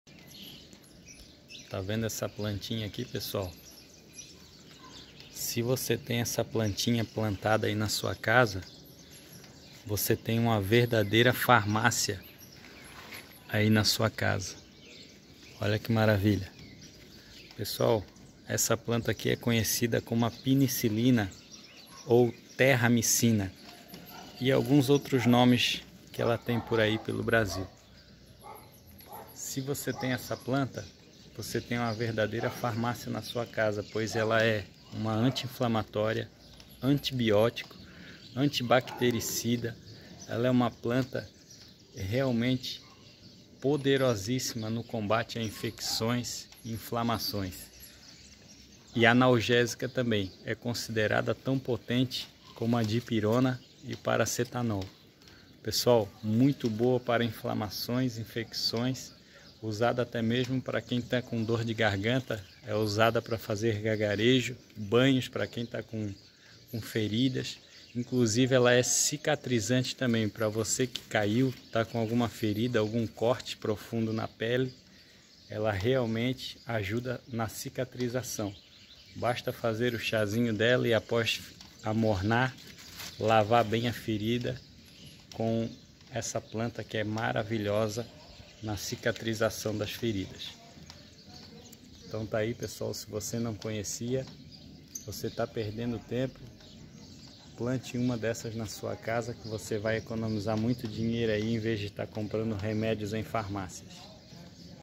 Penicilina uma farmácia natural completa! A Rainha das plantas
terramicina, penicilina uma farmácia natural completa a rainha das plantas
plantas com poder antibióticos  antiflamatorio
plantas medicinais plantas que são usadas como remédio plantas usadas pra infecção e inflamação